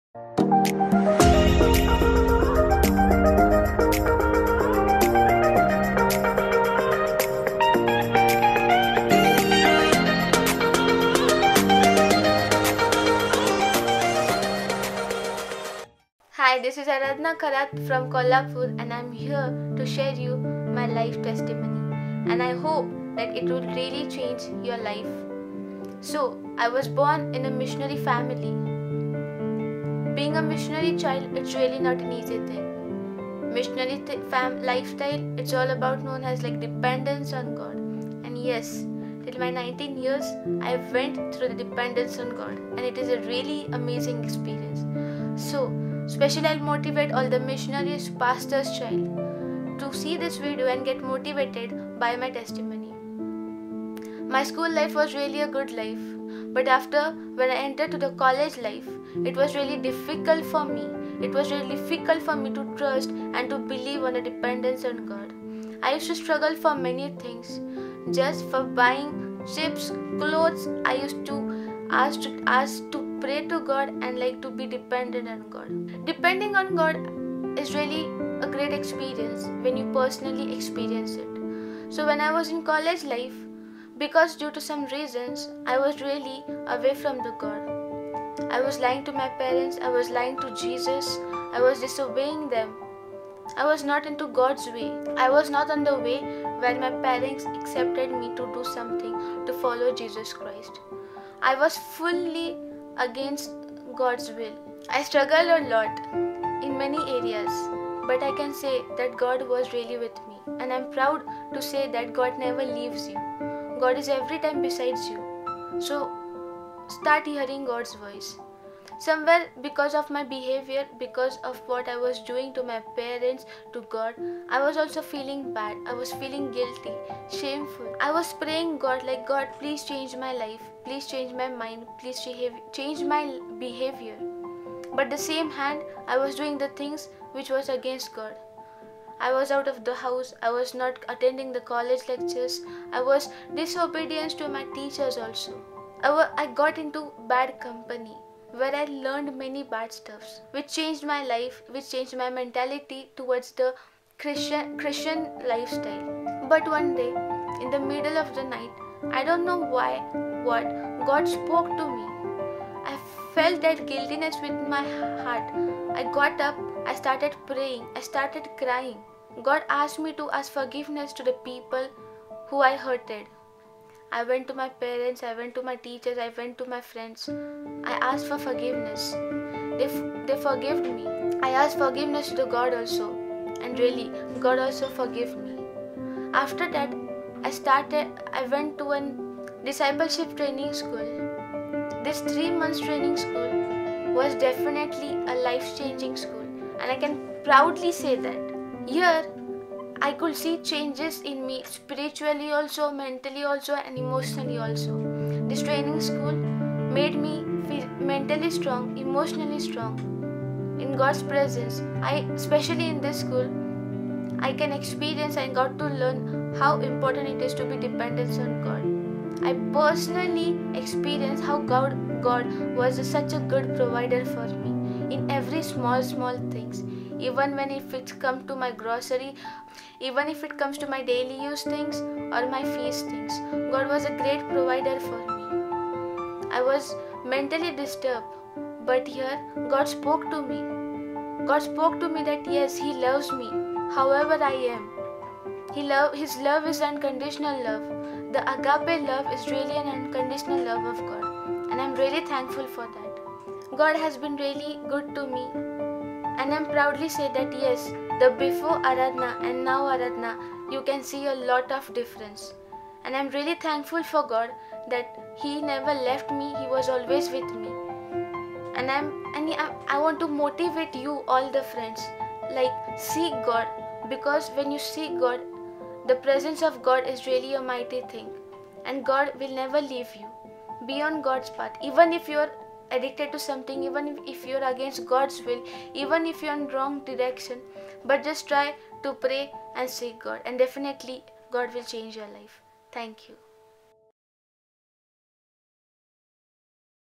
| UNSTOPABLE |Aradhana Kharat | Being a Missionary Child | Testimony | Arise and Shine Worship DTS |
Aradhana Kharat being a missionary kid, brought up by her parents in Kohalpur, Maharashtra. sharing her experience of how difficult it is to be a missionary kid but at the same time it is blessing and privilege.

Produce and Presented by
Arise and Shine  Worship DTS 21'

Programming: Sumit Kale
Arranging and mixing :
Camera : Julia Christina

Sound Space Studio, Lonavala

STAFF TEAM:
Emmanuel
Sumit
Mukilan
Govind
Yohan
Monica
Aksa
Sheetal